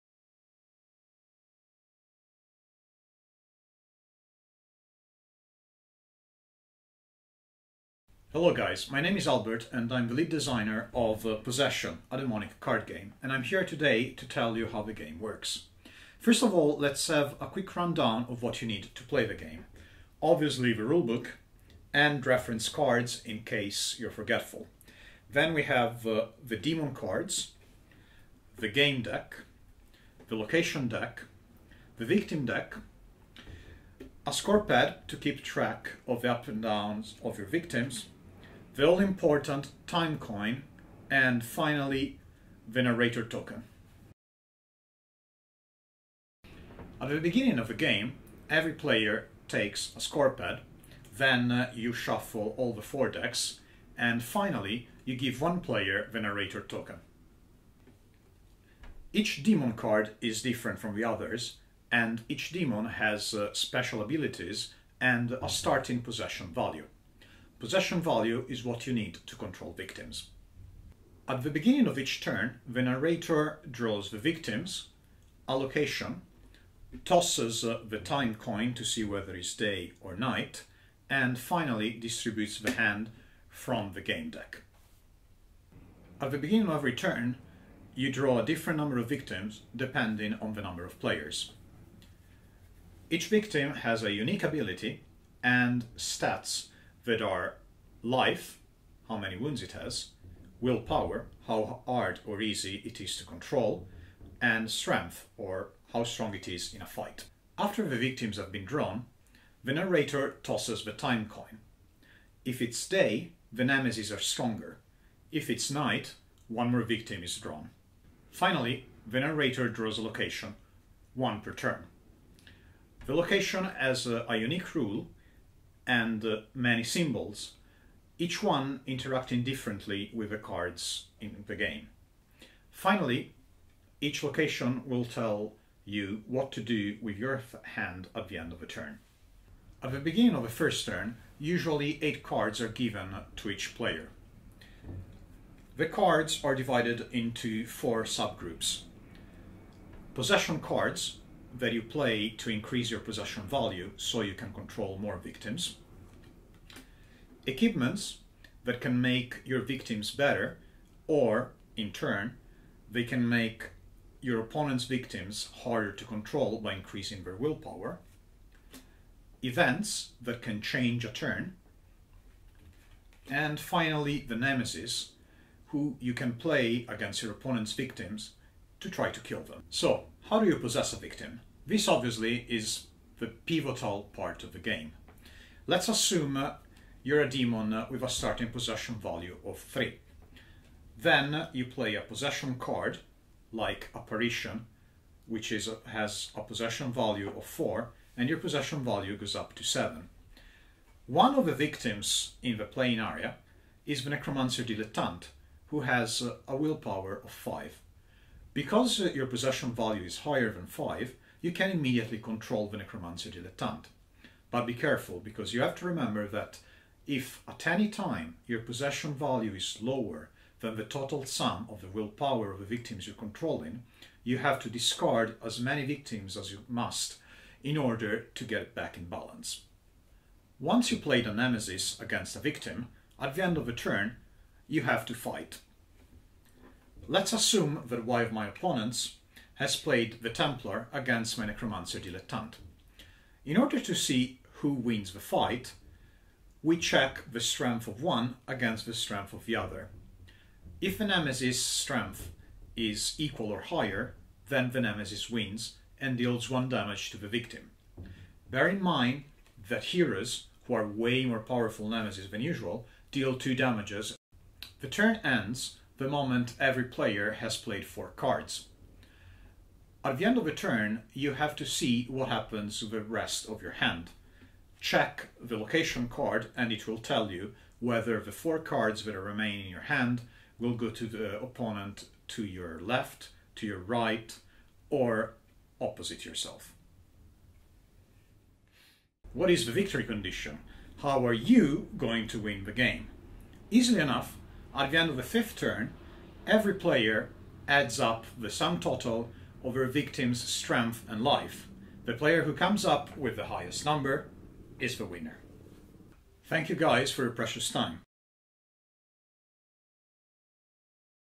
How to play Possession, a Daemonic Card Game
We walk you through how to play Possession, a Daemonic Card Game